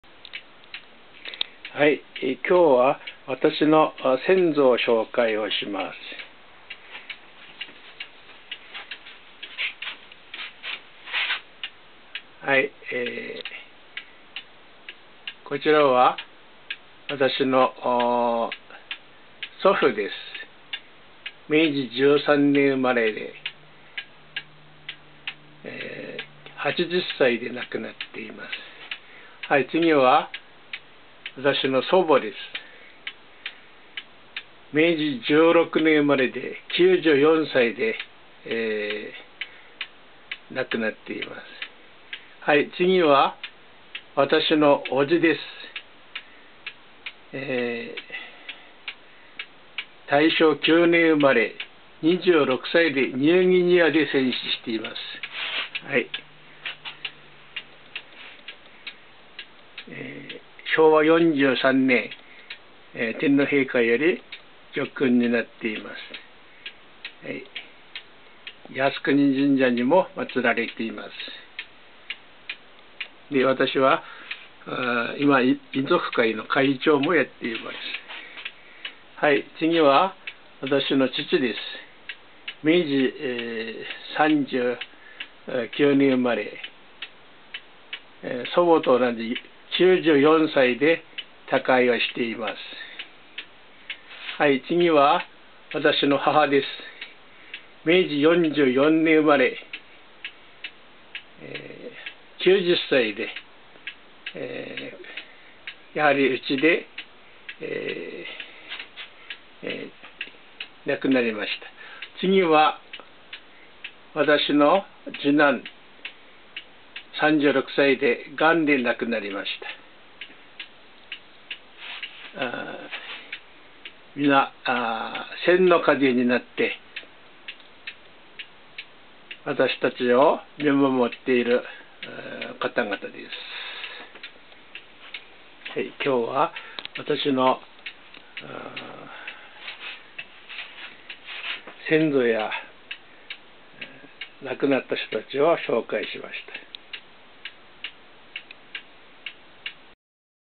我家の千の風の人たち
わが家の黄泉に旅立った人たちです。祖父母、父母は私に健康な体と頭脳のＤＮＡを送ってくれた人であり、若くして戦死した叔父、病死した次男もいます。
　千の風となり日夜私を見守り励ましてくれる人たちに感謝　　早苗